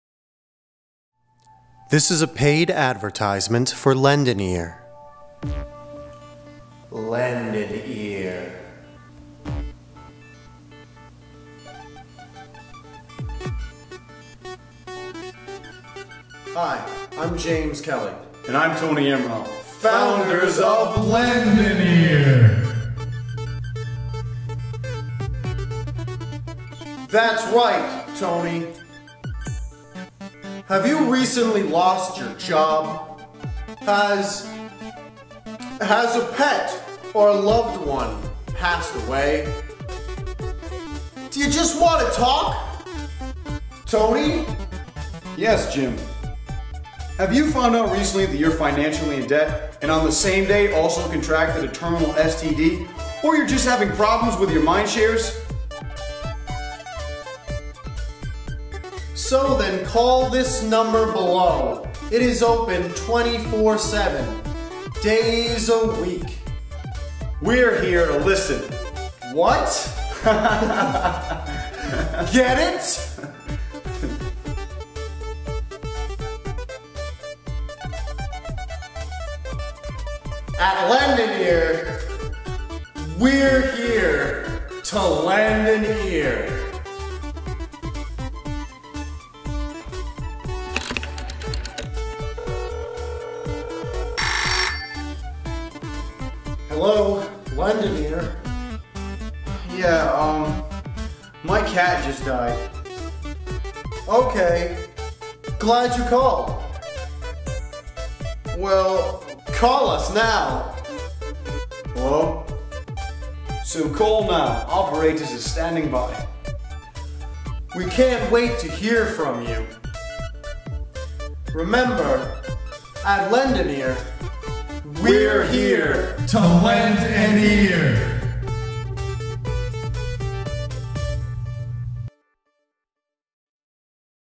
Lend An Ear
Another short that was in our Aug 15th, 2009 show.  This is a business Jim and Tony are trying to start.  Would you call us? The short is supposed to be awkward and the wardrobe changes are inconsistent and this also is obviously planned! This was filmed by Ari Rosenberg.